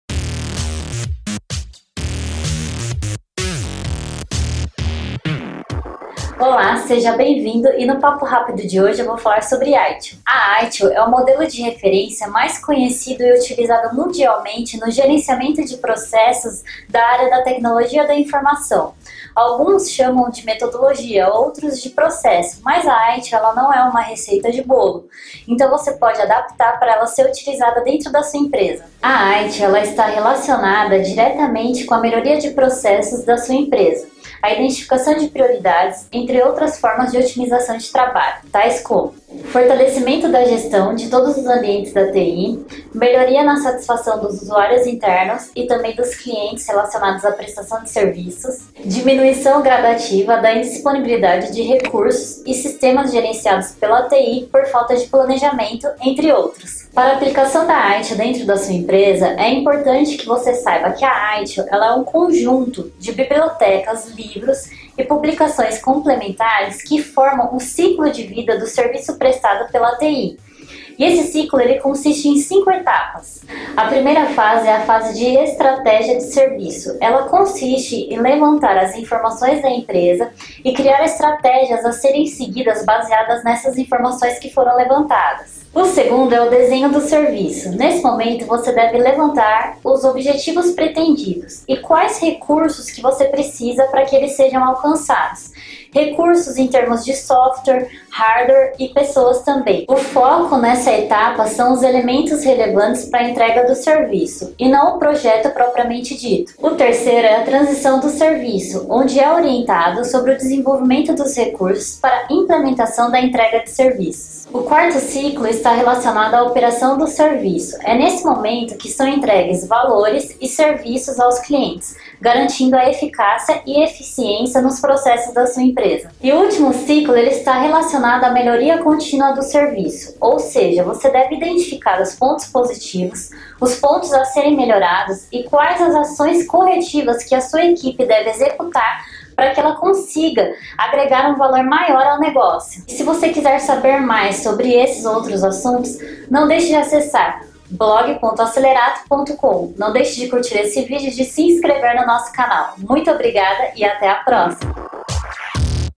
ITIL | Papo Rápido | T2E12
Neste Papo Rápido, Larissa Ramos fala sobre ITIL.

Quer mais dicas para melhorar o seu atendimento? Baixe Grátis o nosso e-book de Guia de Help Desk

A ITIL está diretamente relacionada com a melhoria dos processos de sua empresa, identificação de prioridades, entre outras formas de otimização de trabalho, tais como:

* Fortalecimento da gestão de todos os ambientes da TI;
* Melhoria contínua na satisfação dos usuários internos e clientes com os serviços da TI;
* Redução dos custos Operacionais de TI;
* Orientação a processos com significativa redução nos tempos de execução e distribuição de serviços;
* Diminuição gradativa da indisponibilidade dos recursos e sistemas de tecnologia da informação, causados por falhas no planejamento das mudanças e implantações em TI;
* Reconhecimento da capacidade de gerenciamento pelos acionistas, colaboradores e clientes.

Primeiro, é importante entender que como o próprio nome diz, a ITIL é um Conjunto de Bibliotecas (livros) e outras publicações complementares que formam o Ciclo de Vida do Serviço prestado pela TI. Esse ciclo consiste em 5 fases ou etapas, sendo elas:

* Estratégia do Serviço
* Desenho do Serviço
* Transição do Serviço
* Operação de Serviço
* Melhoria Continuada de Serviço

Para cada empresa, pode ser adotada uma prática diferente para execução de serviços.

Este Papo Rápido é um oferecimento de Acelerato - Gestão de Projetos ágeis e Help Desk
Acesse e descubra como podemos ajudá-lo.